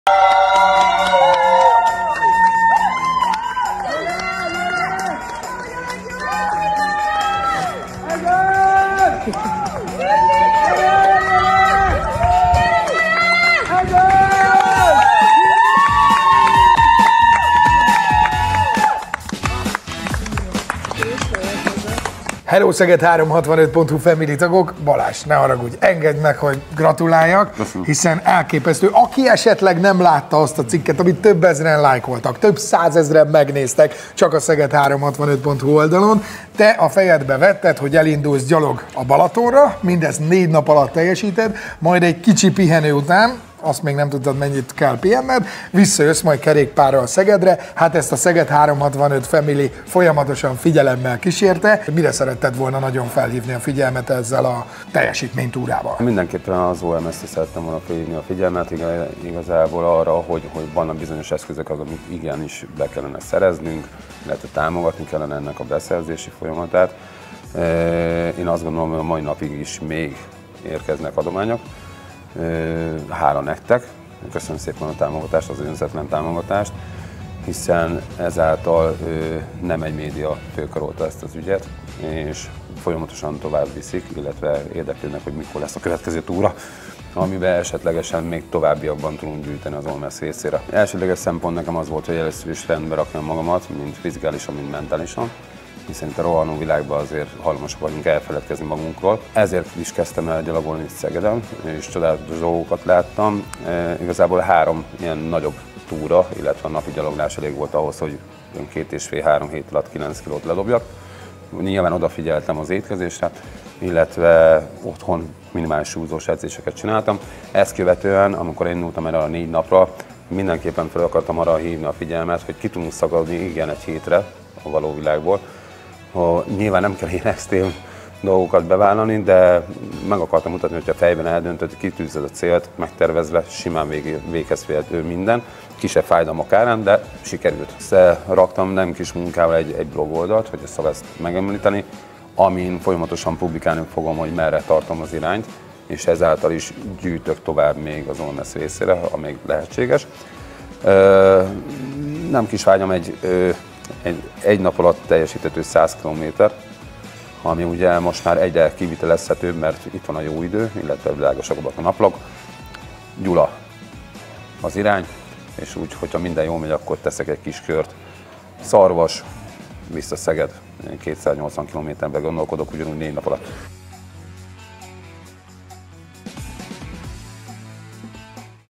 A Szeged365 leste Muráti Balázs minden lábnyomát, aki 4 napig gyalogolt, hogy elérje a Balatont
Muráti Balázst, a Bistorant vezetőjét a sport iránti szenvedély kissé elragadta, mikor 4 nap alatt a Balatonig gyalogolt egyedül. Ezután sem hagyott alább a tűz, hisz visszafelé biciklivel száguldott egészen Szegedig, mindössze egy nap alatt. Ezzel az elképesztő teljesítménnyel nem csupán saját határait feszegette, hanem üzent a világnak, hogy a sport és az aktív életmód igenis mindenkinek való, lehetetlenek pedig nincsenek!